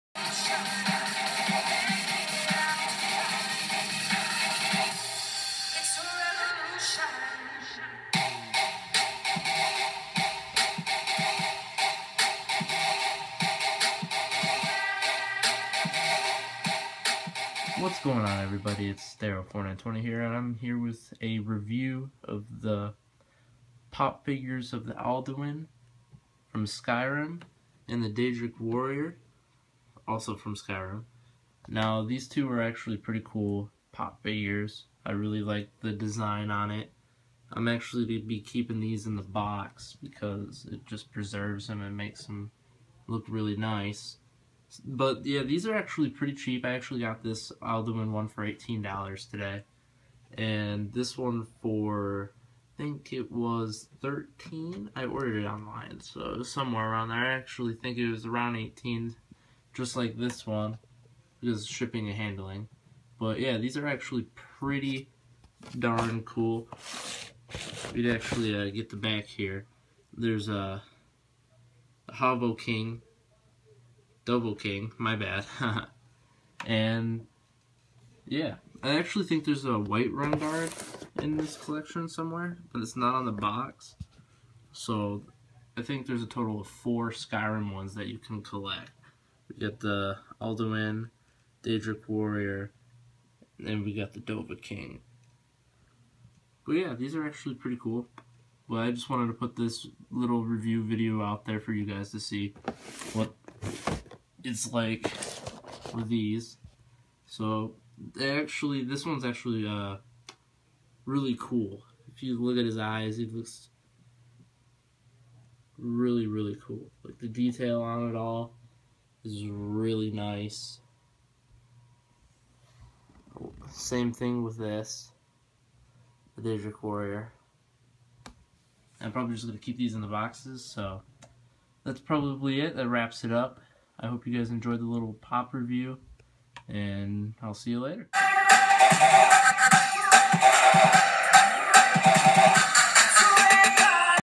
POP! Figures Skyrim edition (Alduin & Daedric warrior) Review!
Just reviewing the POP! Figures I got recently! If you want to shop for these try GameStops website! If you want to see more of these give this video the thumbs up! :)